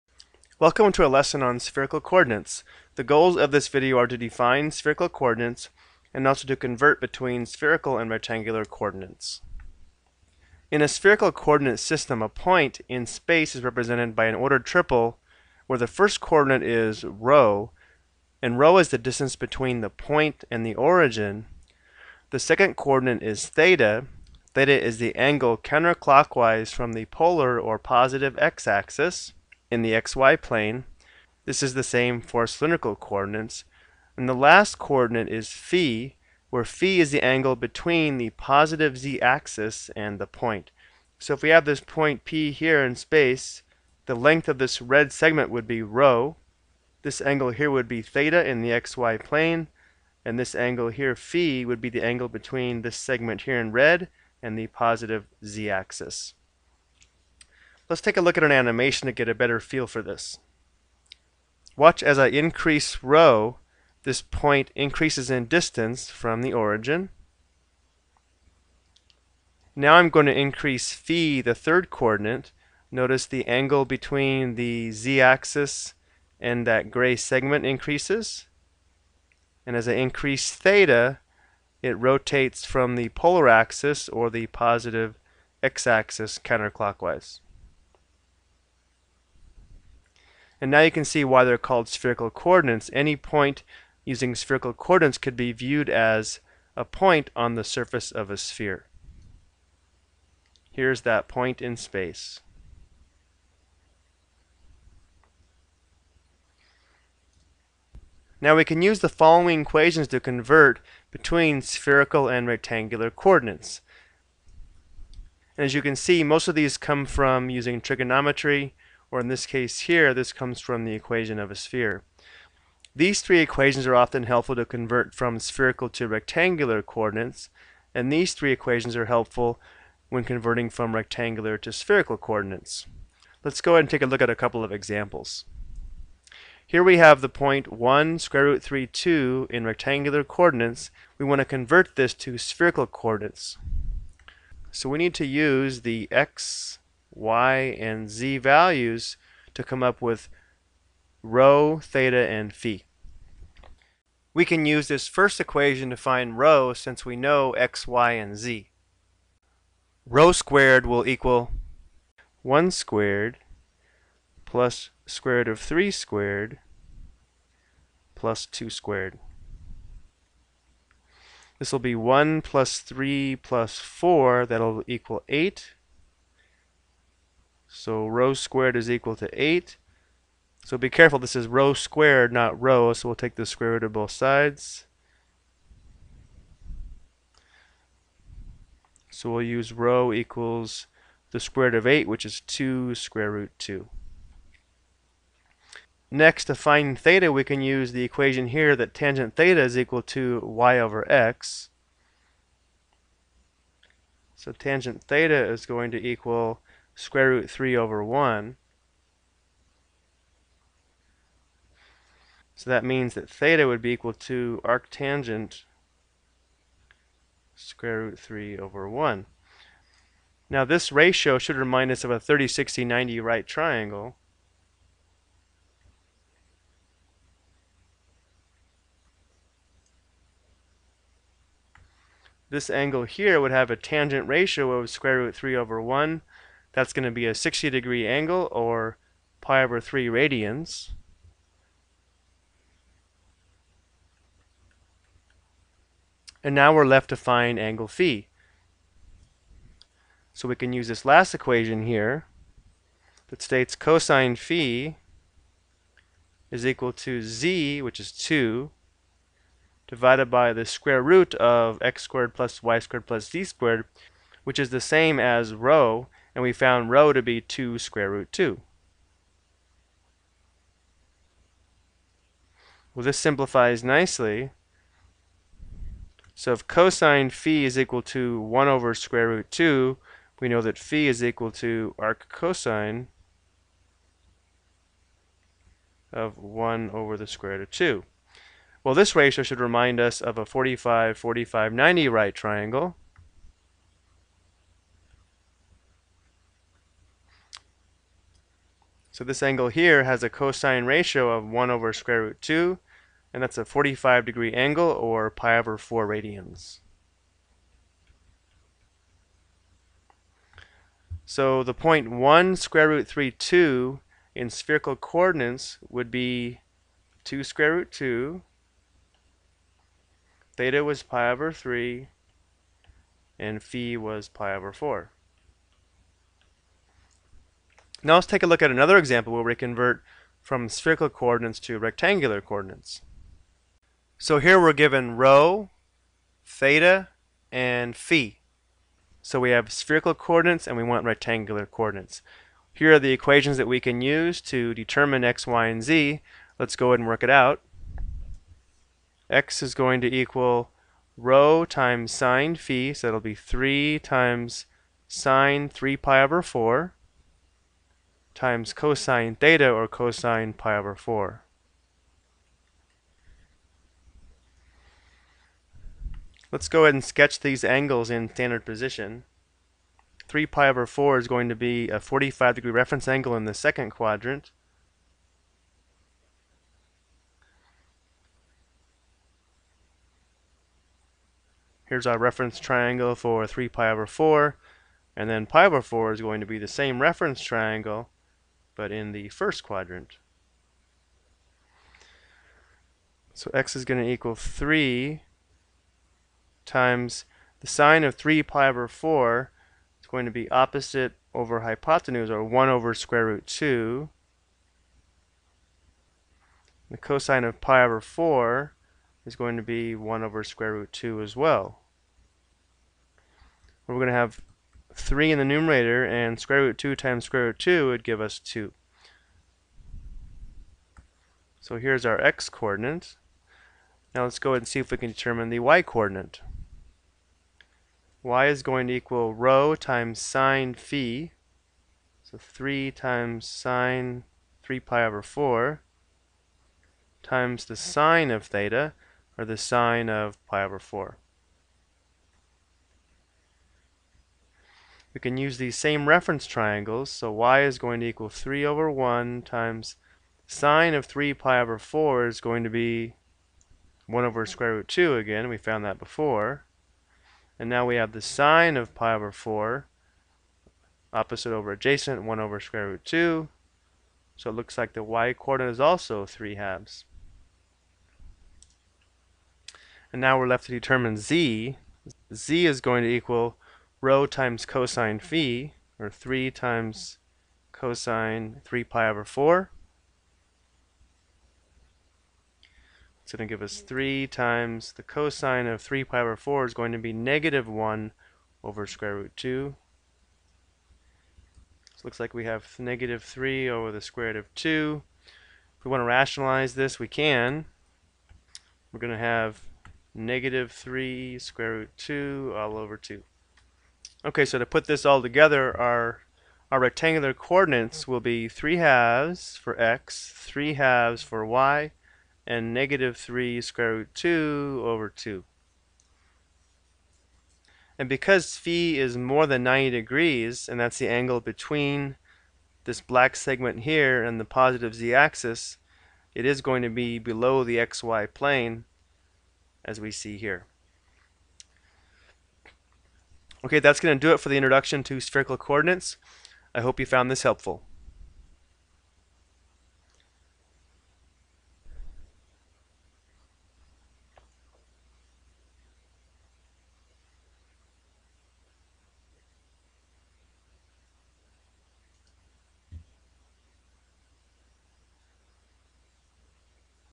Introduction to Spherical Coordinates
This video defines spherical coordinates and explains how to convert between spherical and rectangular coordinates.